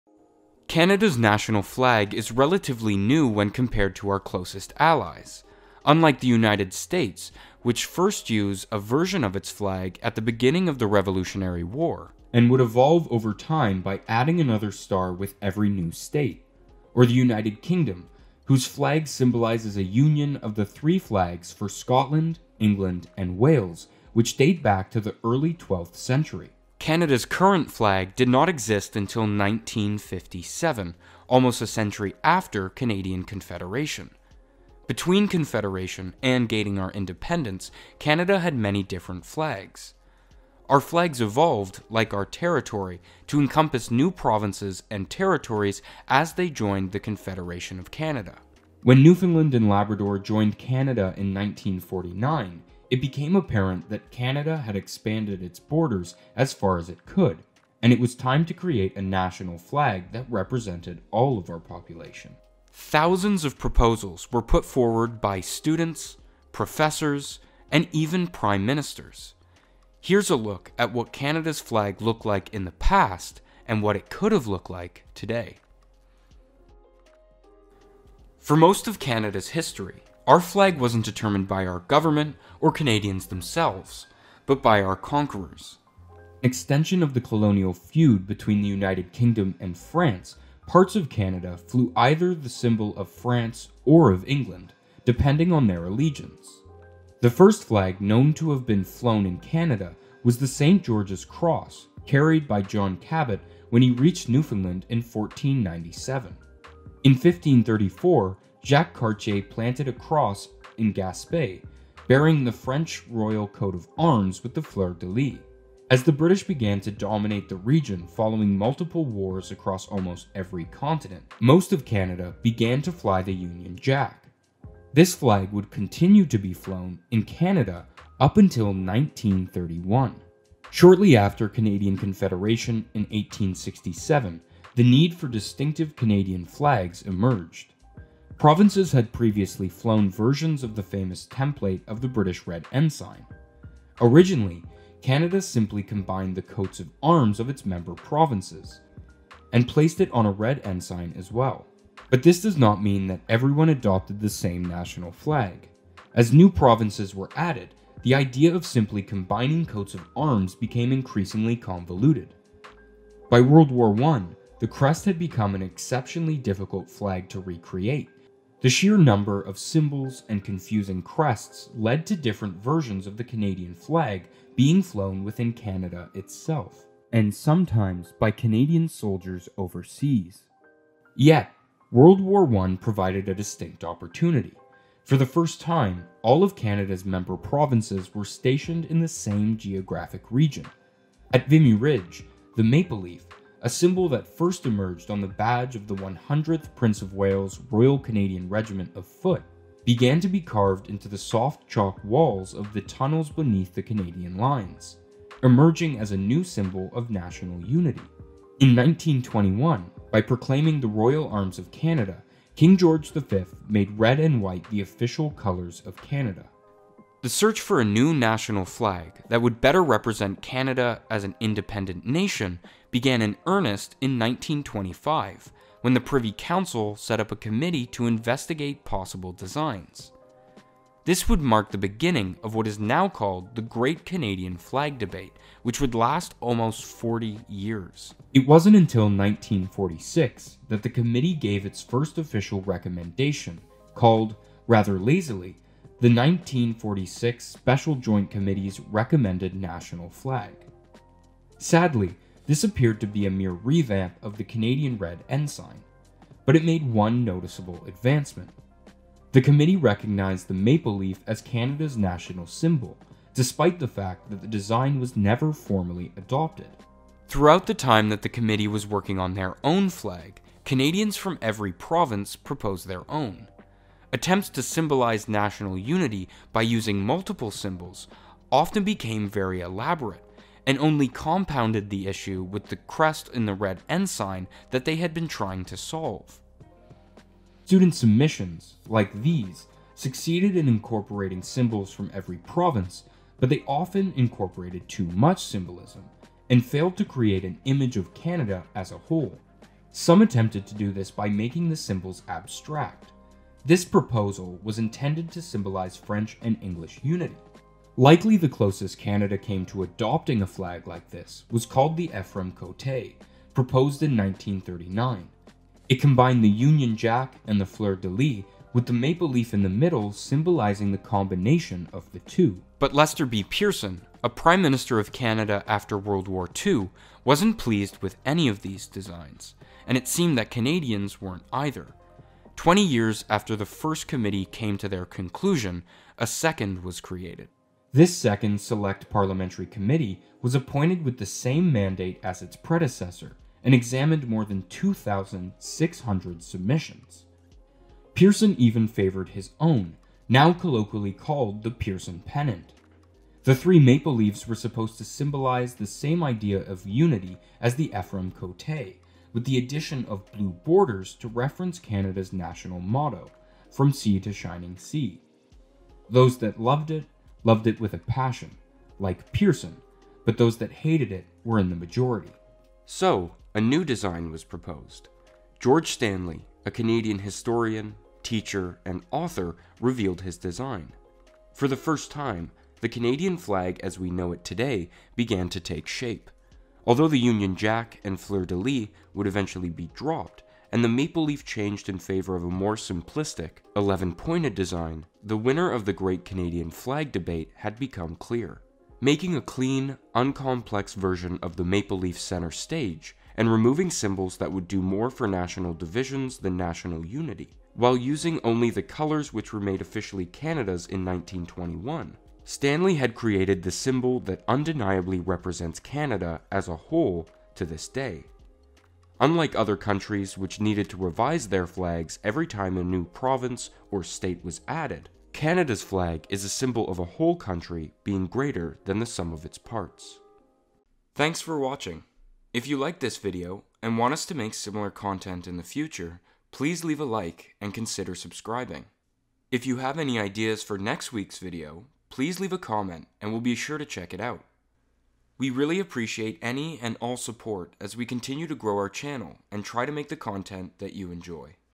How did Canada choose its flag? - Canada Explained
Canada’s current flag did not exist until 1957, almost a century after Canada became a country of its own.

In the time between confederation and full independence, Canada adopted many flags. Like our territory, Canada’s flag continually evolved to symbolize new provinces and territories that had joined confederation.

But, after years of using various versions of the Red Ensign, Canada’s parliament recognized the need for a flag that represented an independent Canada; beginning the 40-year long Great Canadian Flag Debate.